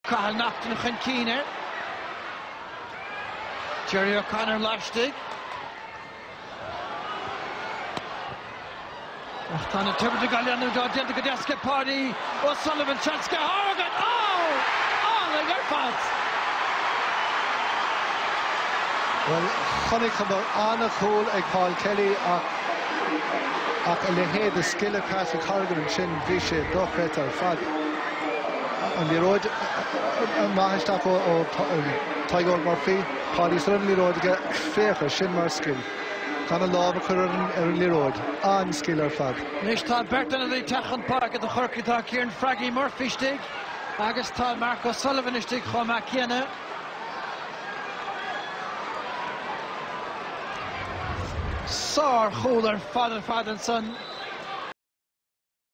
Memorable Pa Horgan goal v Tipp - NHL 2010
A piece of magic from Pa Horgan in this year's national hurling league game vs. Tipperary

patrick horgan cork tipperary gaa hurling goal national league brendan cummins premier rebels